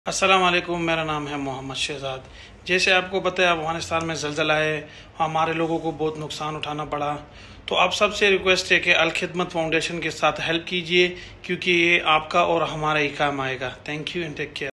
Afghan Cricketer Muhammad Shehzad requests donations for Afghanistan
Afghani cricketer and wicketkeeper requests donations to help the earthquake-stricken people of Afghanistan. Our brothers are in desperate need for food and medical aid so please donate as much as possible and help Alkhidmat save as many lives as possible📦⛑️🏥!